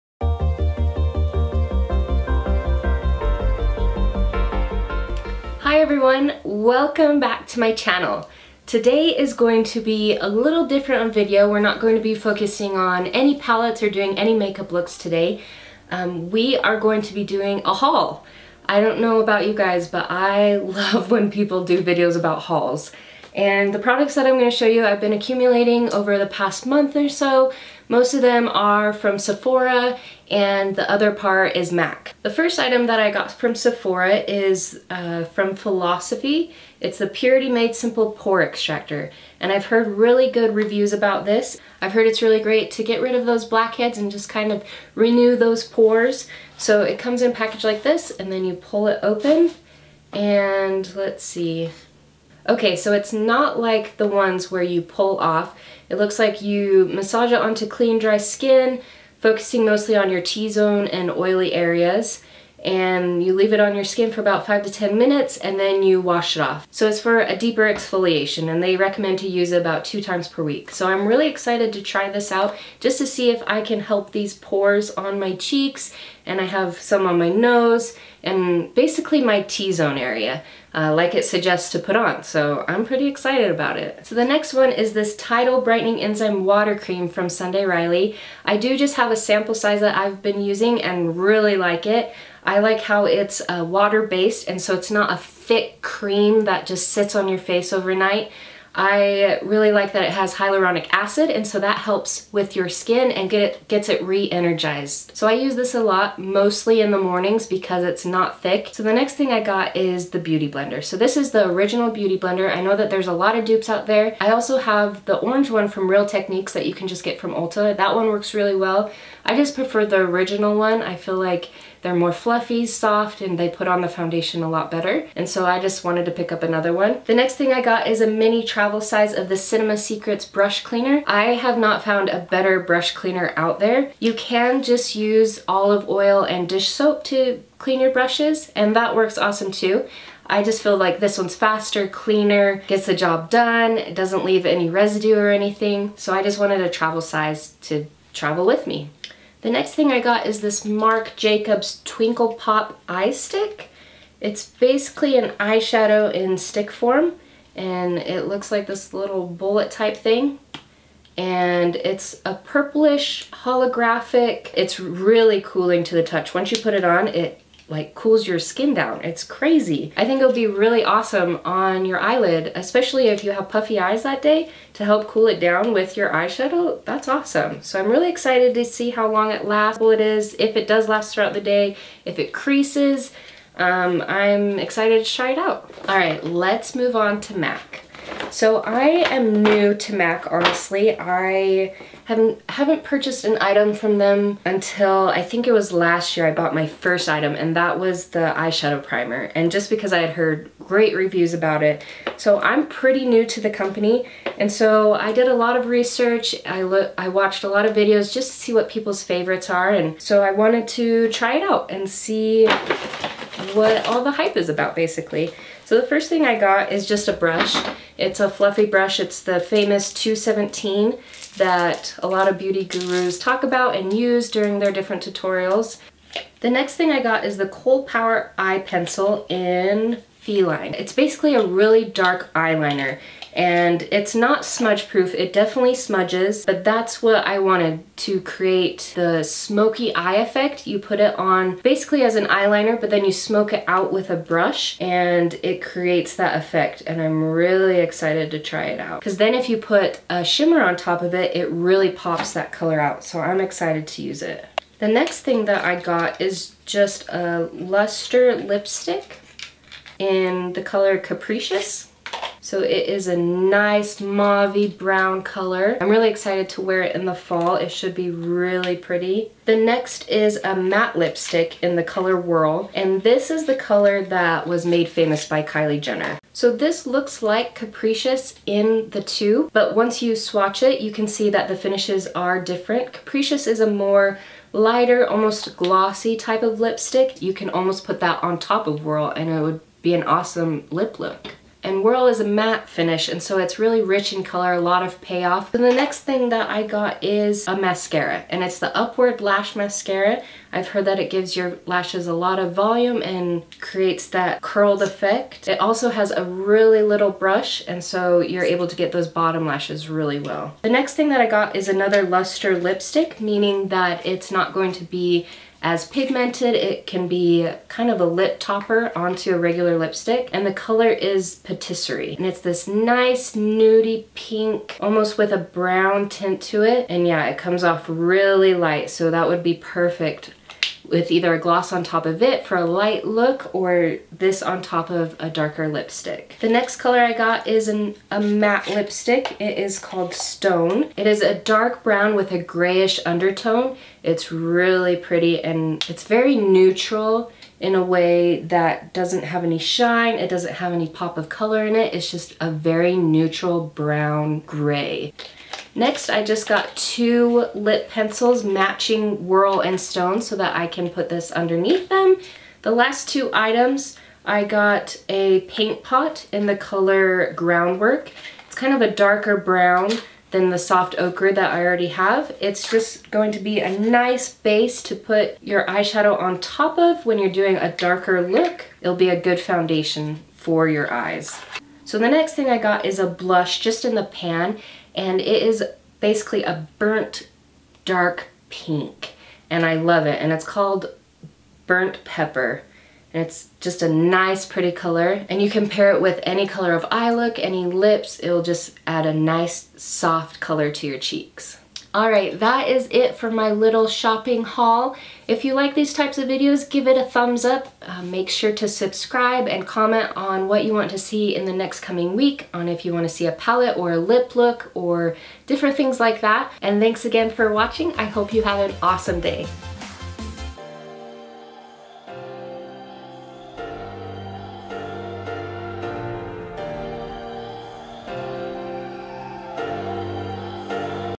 SEPHORA & MAC HAUL || TAVVVLES
Today's video is a bit different! It's a HAUL! I'm a sucker for a good haul and I hope you are too! Let me know if there are any products that you want me to try out in a video!
Also I definitely am not good at describing colors haha!

▷ PRODUCTS MENTIONED ‣
|SEPHORA|
-Philosophy - Purity Made Simple Pore Extractor
-Sunday Riley - Brightening Enzyme Water Cream
-Cinema Secrets - Brush Cleaner
-Marc Jacobs - Twinkle Pop- On The Verge
-Beauty Blender

|MAC|
-Blending Brush - 217
-Kohl Liner - Feline
- Lustre Lipstick - Capricious
-Matte Lipstick - Whirl
-Lustre Lipstick - Patissiere
-Matte Lipstick - Stone
-Paint Pot - Groundwork
-Blushes - Burnt Pepper

▷ PRODUCTS USED ON MY FACE ‣
MAKE UP FOR EVER - Primer- Step 1 Skin Equalizer Primer
Urban Decay - Primer- Self-Adjusting Complexion Primer
IT COSMETICS - Concealer- Bye Bye Redness
TARTE - Concealer - Rainforest of the Sea
TARTE - Foundation - Amazonian Clay
Coty Airspun - Translucent Loose Setting Powder

E Y E B R O W S ⋆
Benefit Cosmetics - ka-BROW! Cream-Gel Eyebrow Color

NARS - Contour - Laguna
BENEFIT COSMETICS - Blush - Dandelion
Sleek Cosmetics - Highlight Palette

URBAN DECAY- Eyeshadow Primer- Eden
DOSE OF COLORS - Eyeshadow Palette- Baked Browns
MAYBELLINE- Mascara- Colossal Big Shot
HOUSE OF LASHES - Strip Lashes- Boudour

DOSE OF COLORS - Liquid Lipstick -

BRUSHES ⋆
All of the Brushes I used were from morphebrushes.com